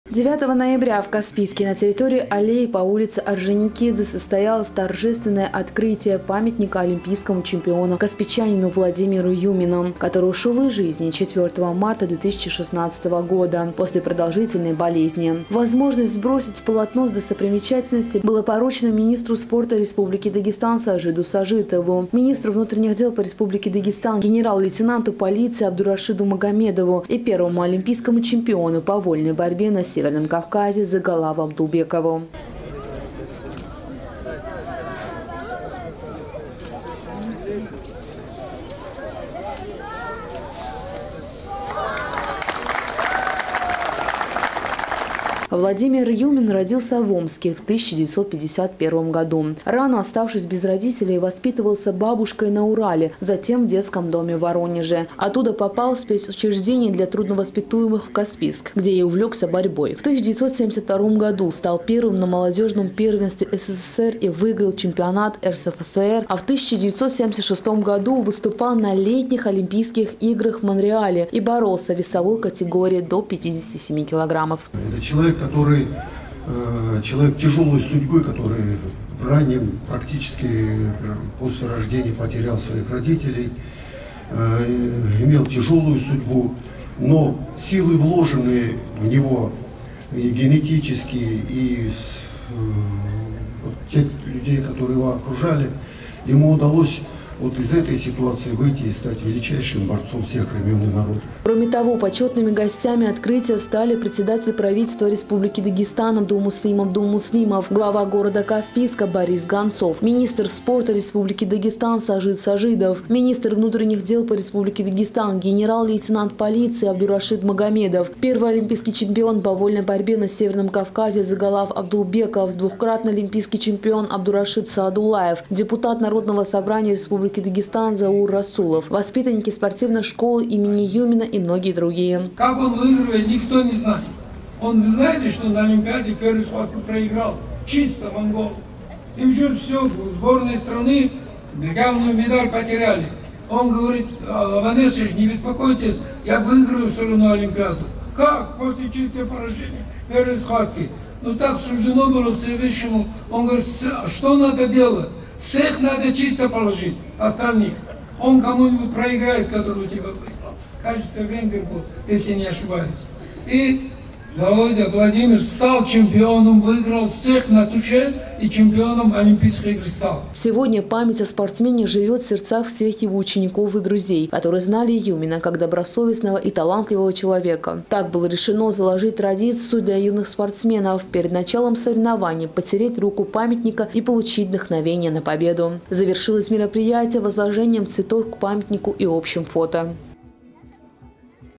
Памятник Владимиру Юмину открыли в Каспийске
9 ноября в Каспийске на территории аллеи по ул. Орджоникидзе состоялось торжественное открытие памятника олимпийскому чемпиону, каспийчанину Владимиру Юмину, который ушел из жизни 4 марта 2016 года после продолжительной болезни. Возможность сбросить полотно с достопримечательности было поручено министру спорта РД Сажиду Сажидову, министру внутренних дел по РД, генерал - лейтенанту полиции Абдурашиду Магомедову и первому олимпийскому чемпиону по вольной борьбе на северном Кавказе Загалаву Абдулбекову.

Владимир Юмин родился в Омске 1951 года. Рано оставшись без родителей, воспитывался бабушкой на Урале, затем в детском доме в Воронеже. Оттуда попал в спецучреждение для трудновоспитуемых в Каспийск, где и увлёкся борьбой. В 1972 году стал первым на молодежном первенстве СССР и выиграл чемпионат РСФСР, а в 1976 году выступал на летних олимпийских играх в Монреале и боролся в весовой категории до 57 килограммов.